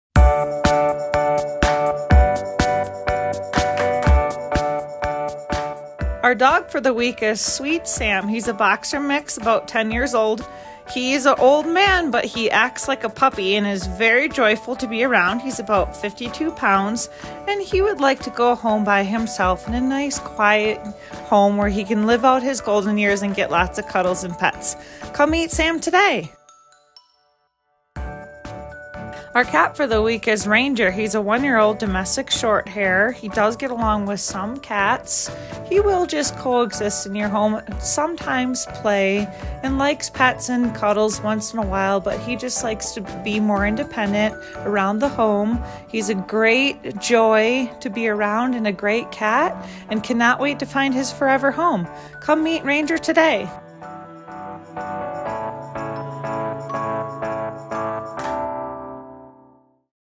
Pets of the Week for October 20th, 2021
These two boys are this week's featured pets and are eager to find their forever homes. Learn more about them at the Summit County Animal Shelter.